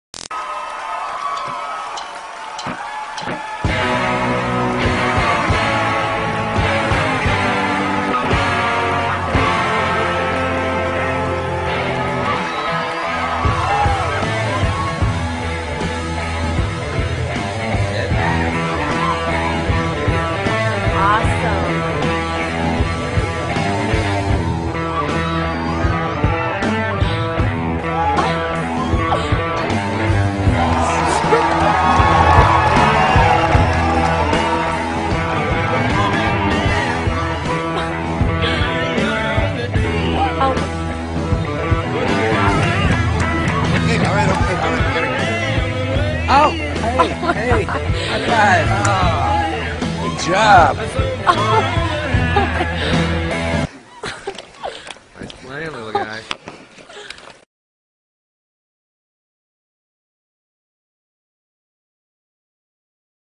BABY👶 PLAYING 🎸GUITAR || LIKE A PRO
Baby guitarist playing guitar better then professionals